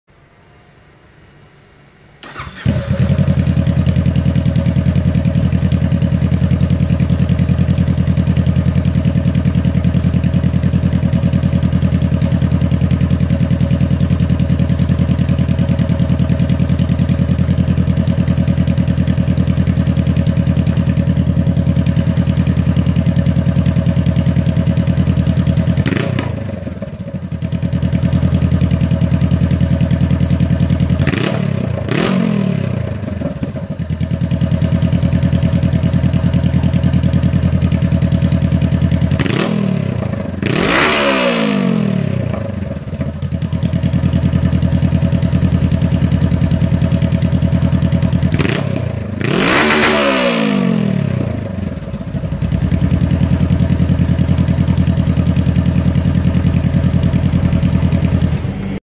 2014 Honda cbr500r exhaust
Straight pipe. $5 clamp on tip from O'Reillys. Recorded on a Samsung Galaxy s4. The exhaust was aimed at my garage so it sounds a tad different in person but overall you get the point.